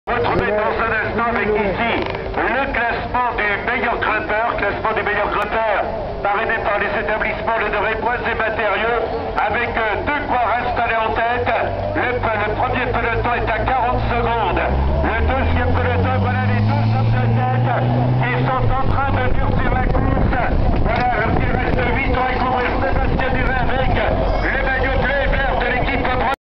grand prix de plumelec 2010 - un premier peloton
a environ 10 tours de l'arrivée,,, un premier peloton emmené par dimitri champion